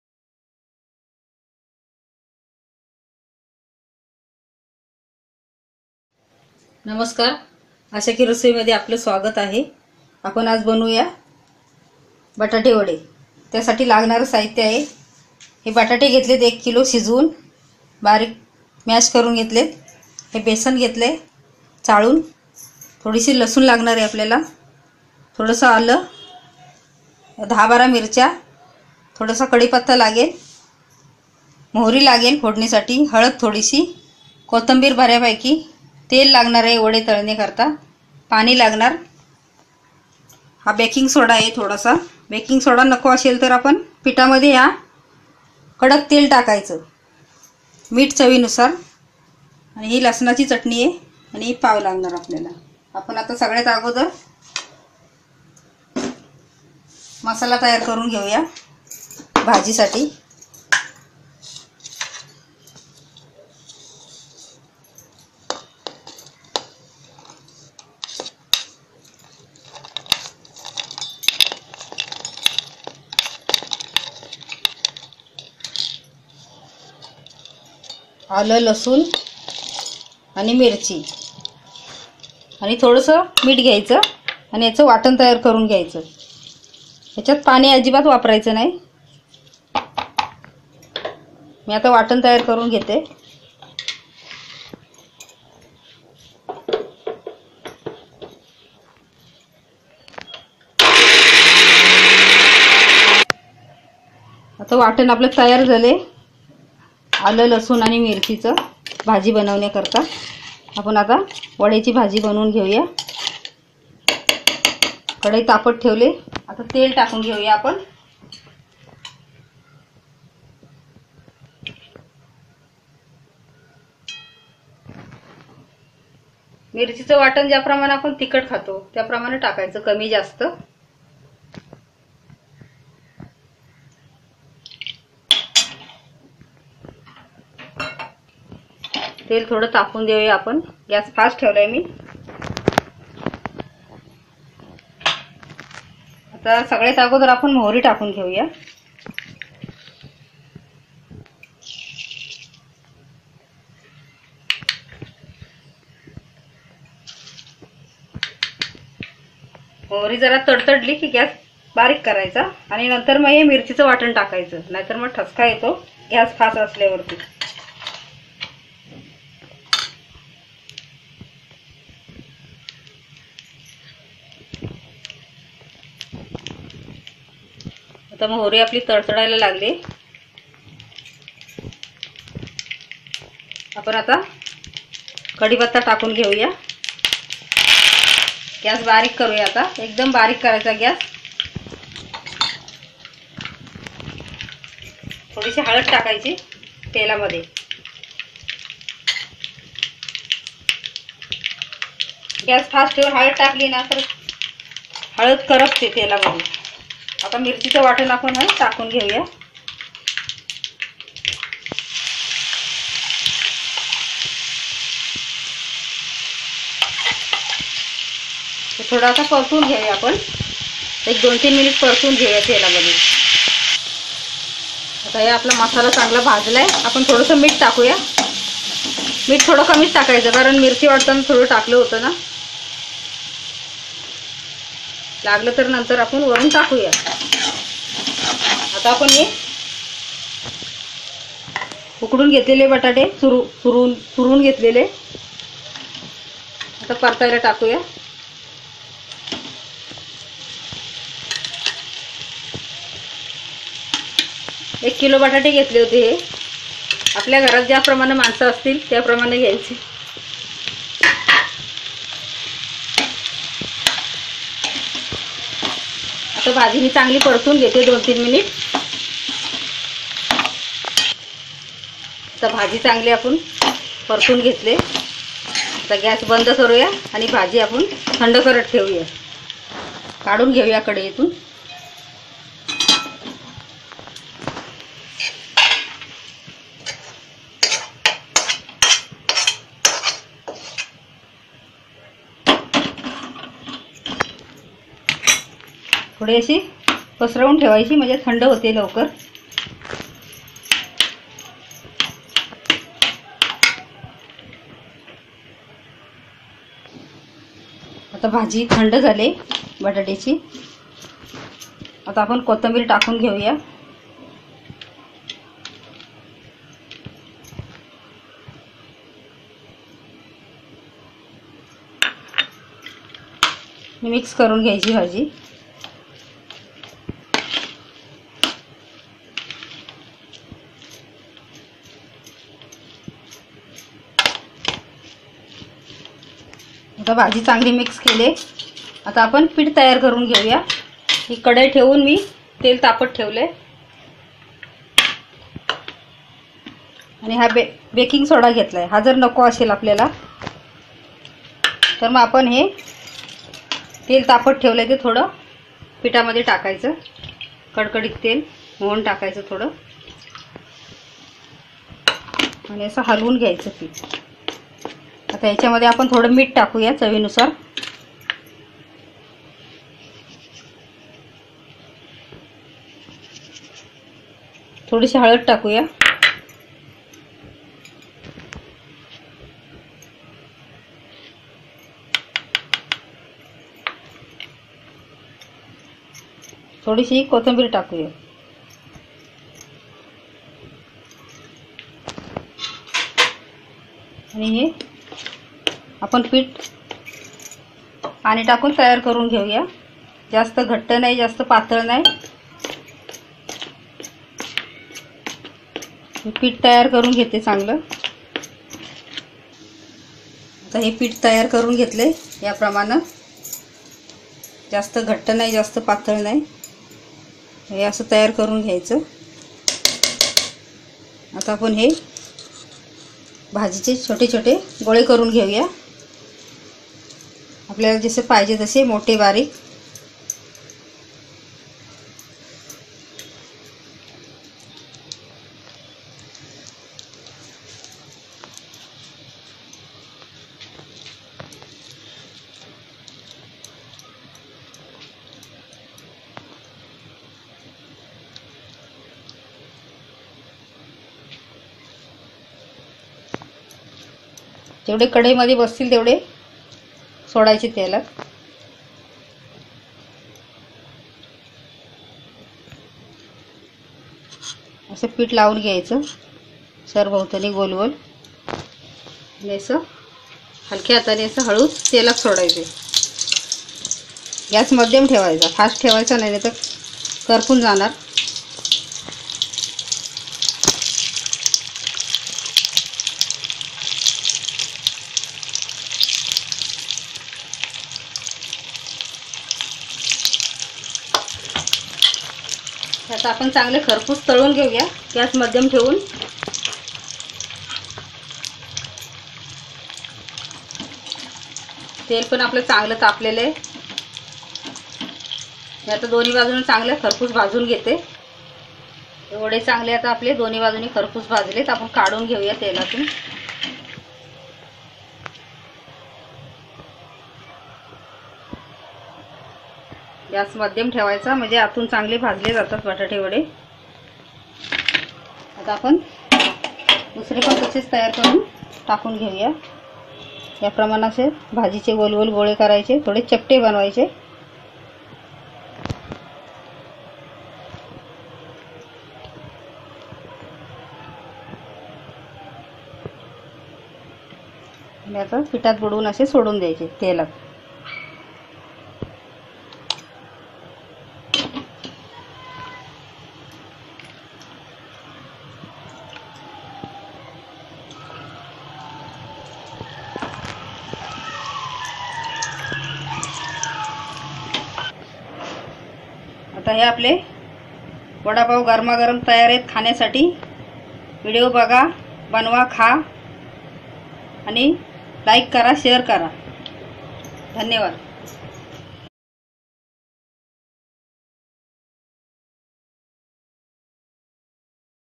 VADA PAV | वडा पाव | Special Maharashtrian Food In Marathi By Asha Maragaje
Watch And Learn How To Make Quick Maharashtrian Recipe Vada Pav In Marathi With Asha Maragaje